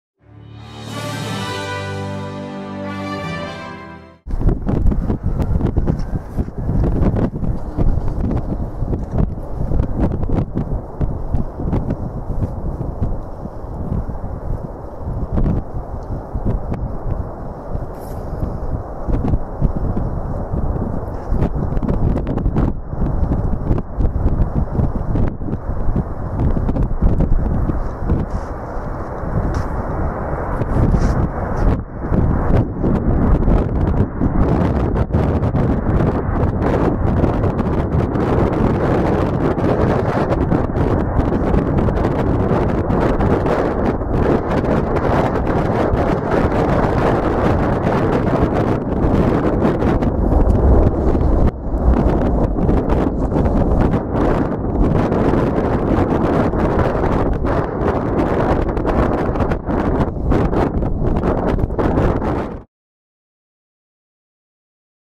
KLM A330 taking off from Calgary
KLM Royal Dutch Airlines Airbus A330 taking off from Calgary International (CYYC/YYC), bound for Amsterdam Schiphol (EHAM/AMS).